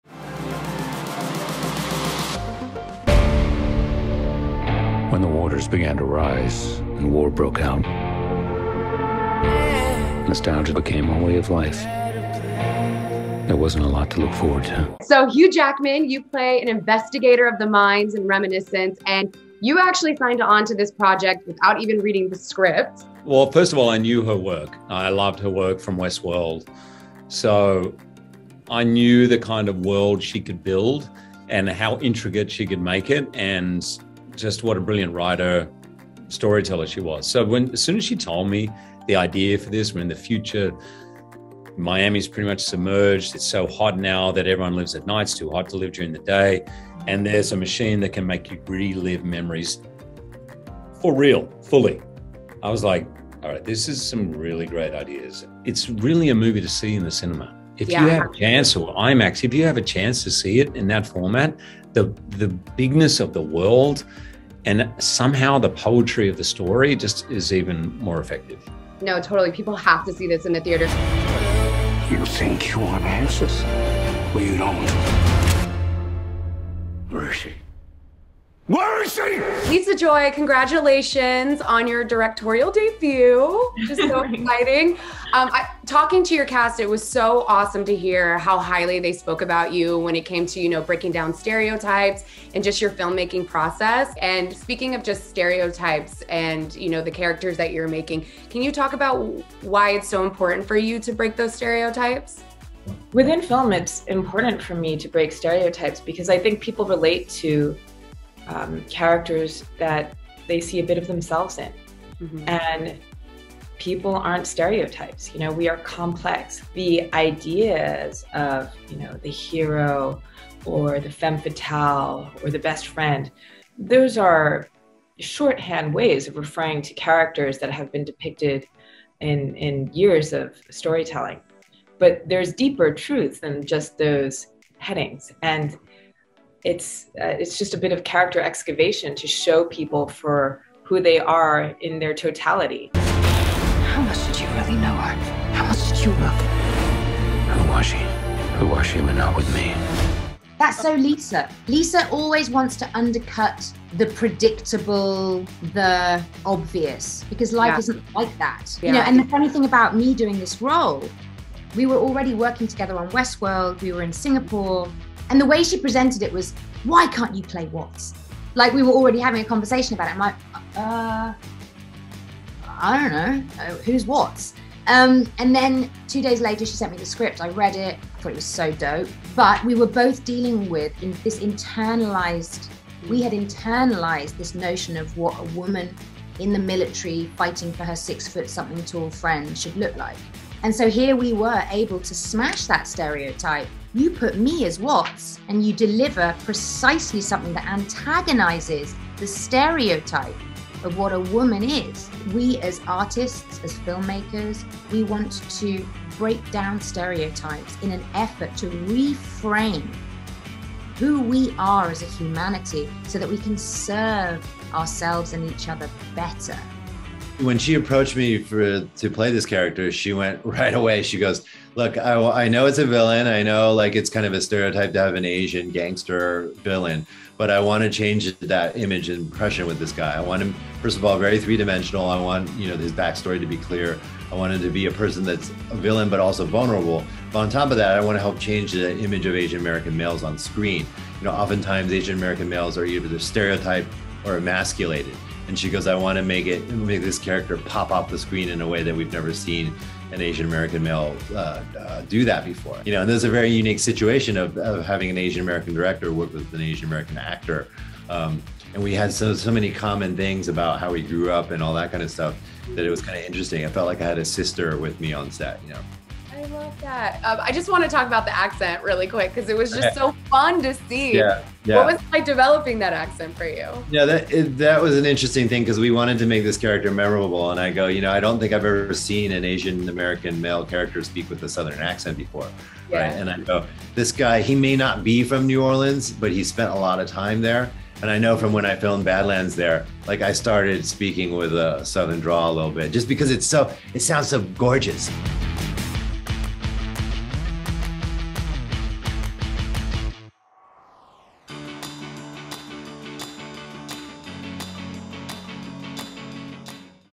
Sit Down With the Stars of Reminiscence! Feat. The Cast and Crew
The past is never over. Watch Jackie Iadonisi chat with the cast and crew of Reminiscence in this exclusive interview! Catch the movie at Regal on August 20, 2021.

A scientist discovers a way to relive your past and uses the technology to search for his long lost love.

Director: Lisa Joy
Writer: Lisa Joy
Stars: Hugh Jackman, Rebecca Ferguson, Thandiwe Newton, Natalie Martinez